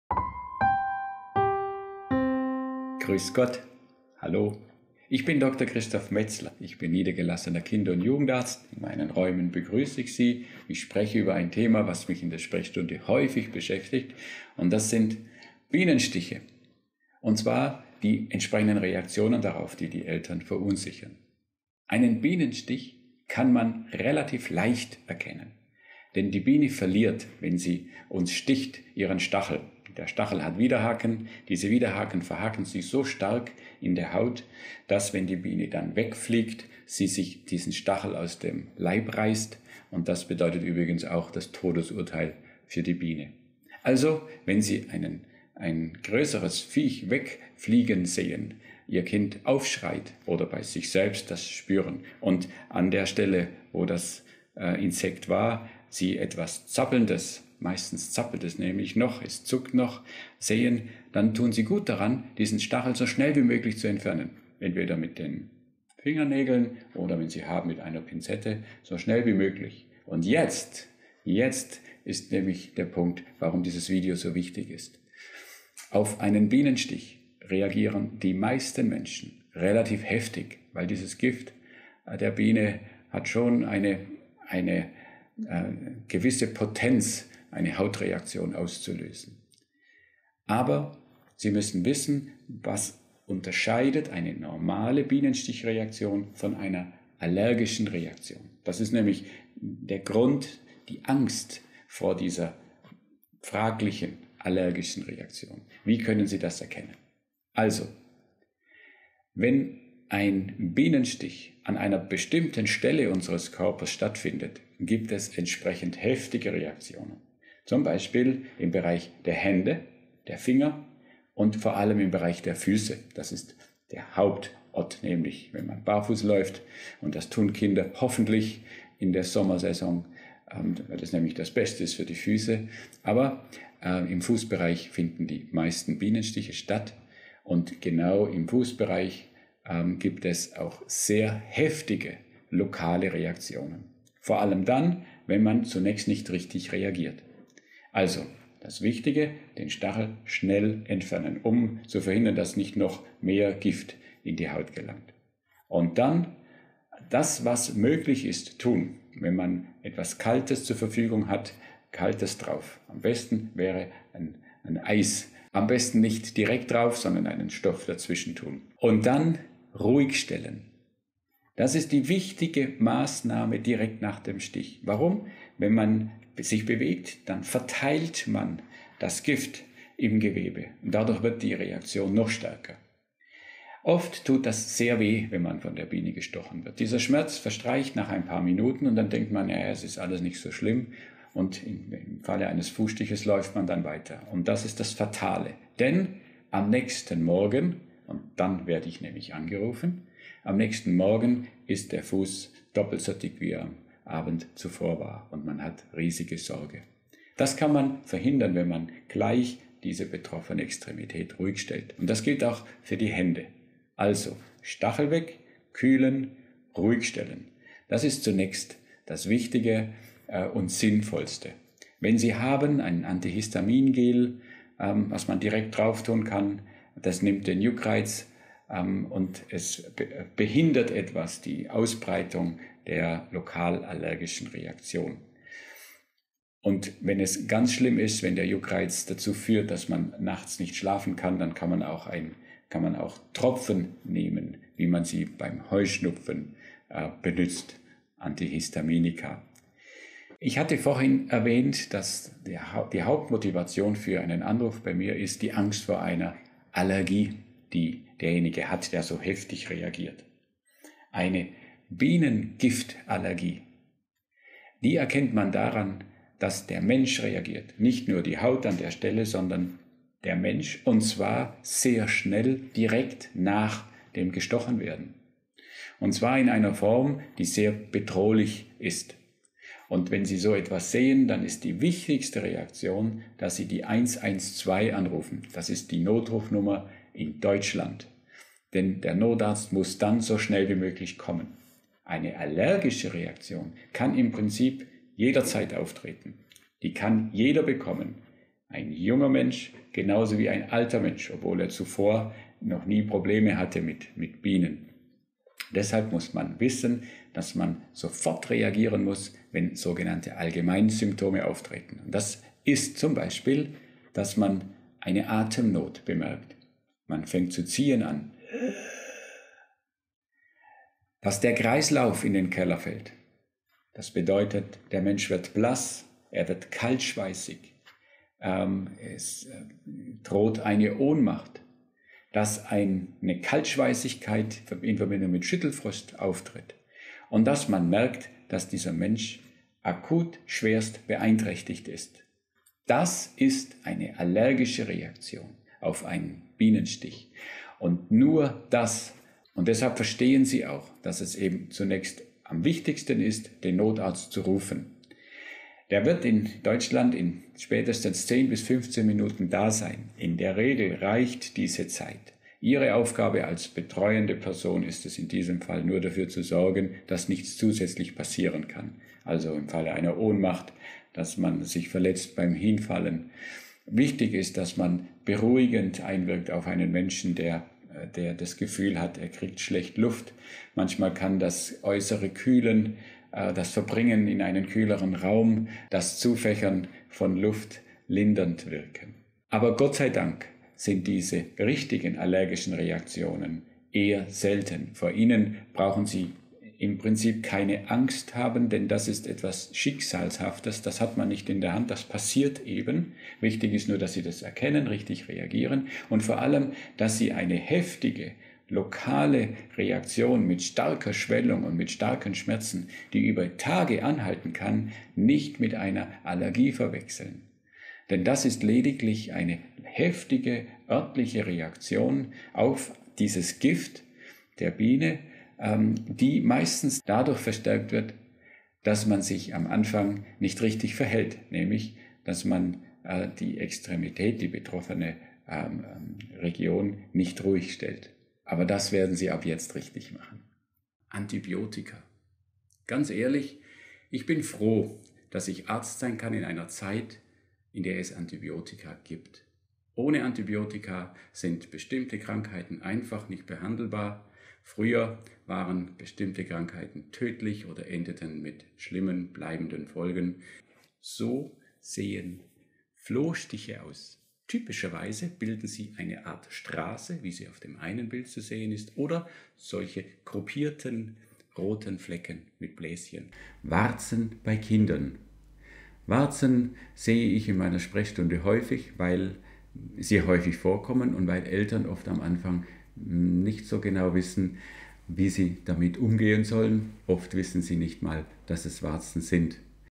Bienenstiche - Der Kinderarzt vom Bodensee
Aus seiner persönlichen und seiner Sprechstundenerfahrung heraus spricht der Kinder- und Jugendarzt Dr. med. Christof Metzler über Bienenstiche, die Bienengiftallergie und wie Sie richtig reagieren, wenn der Notfall eintritt. Auch erklärt er, wie Sie die Schwellung der gestochenen Stelle einschränken und woran Sie einen Bienenstich erkennen.